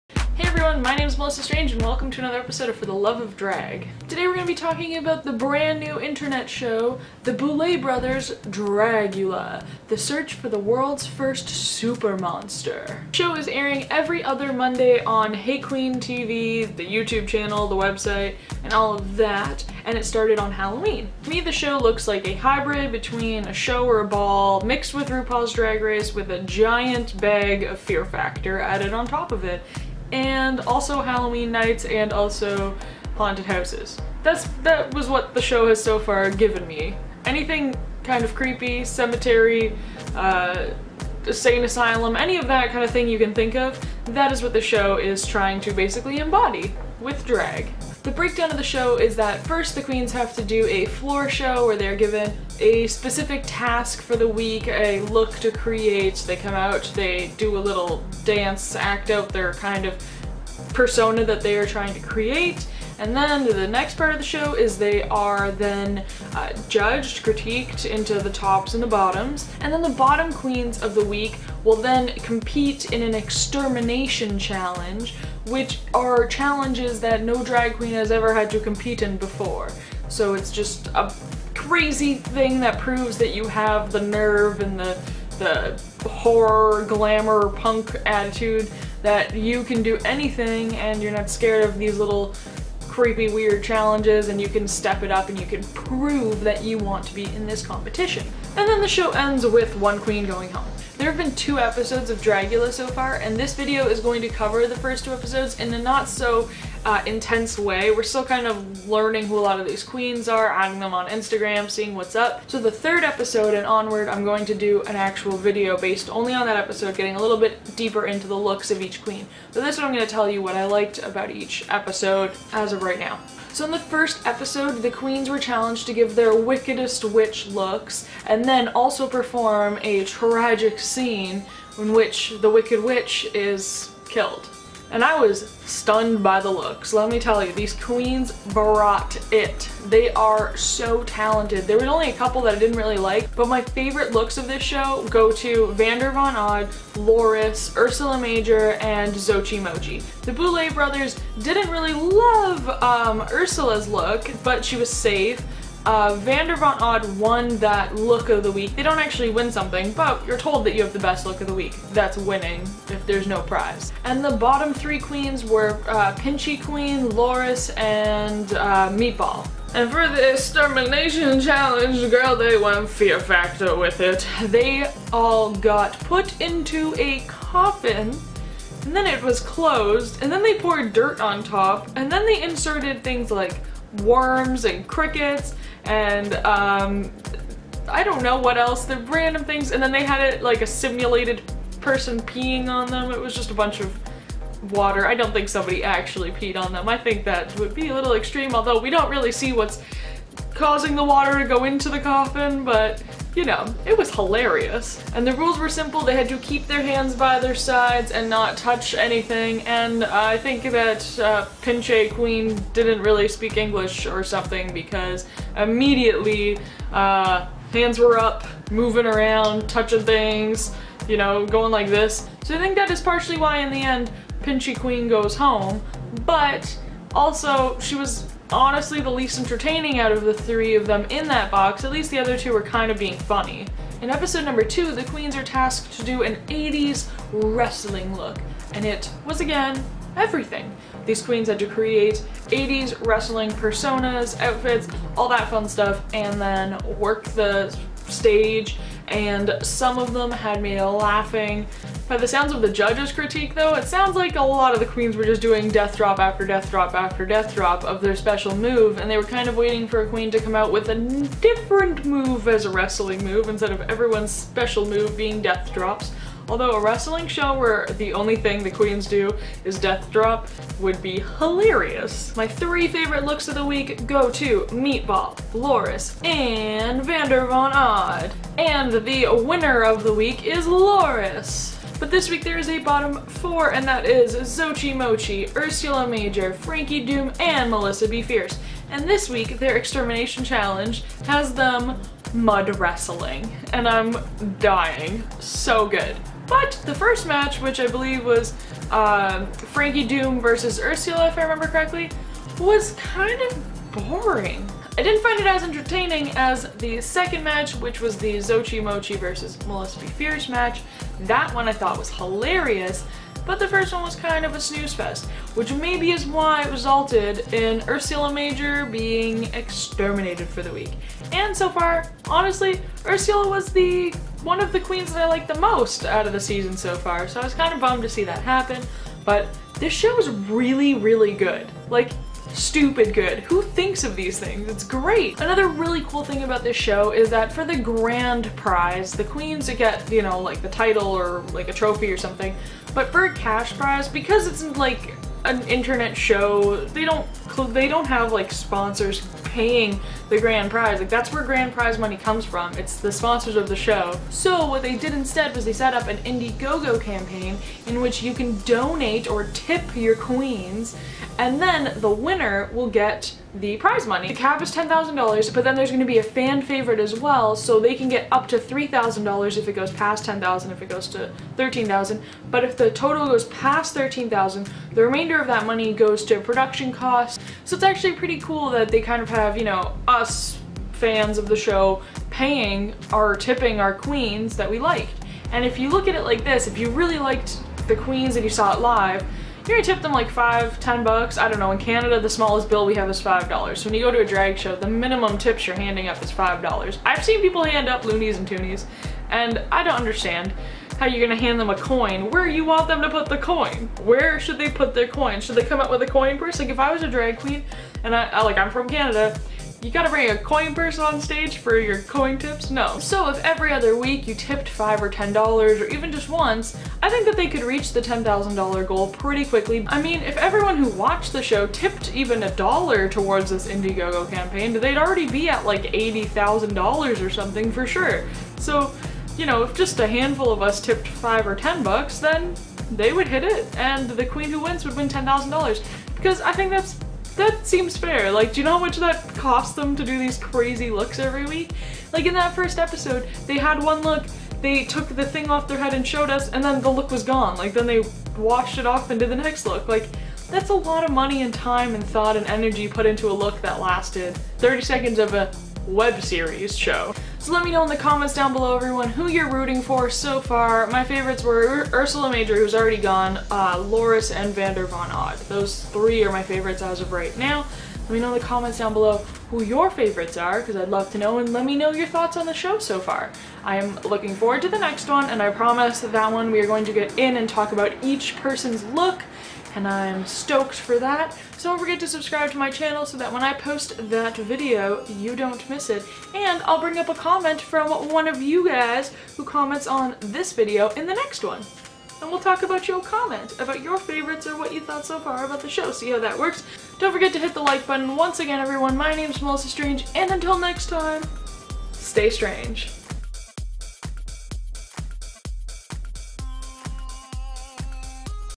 THE BOULET BROTHERS DRAGULA RECAP & REVIEW || EP1 & EP2
The Boulet Brother DRAGULA the search for the world's first drag supermonster is in full swing! With 2 episodes currently available I figured it was time to share with you all my opinion, thoughts, and predictions as well as a short recap of the first 2 episodes :)

NEW VIDEOS: TUESDAY & THURSDAYS &/OR FRIDAYS

Comments are cool!